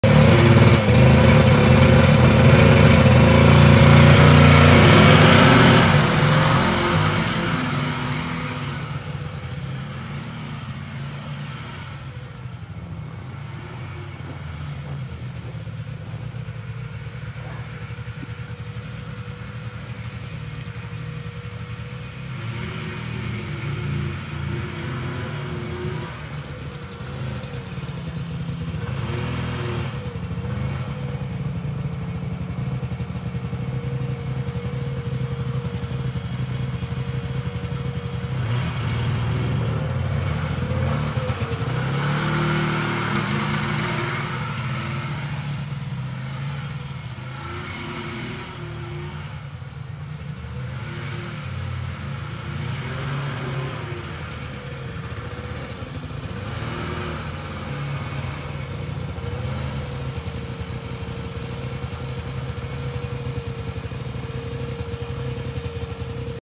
Night time ride on the 1974 John Deere JDX6
Riding these old vintage snowmobiles at night is quite fun and adventurous, they actually have pretty good lighting for being 50 years old, we upgraded the voltage regulators and headlamps to a more modern version and they light up the trail very nicely as long as you don’t go too fast and overdrive the headlight😅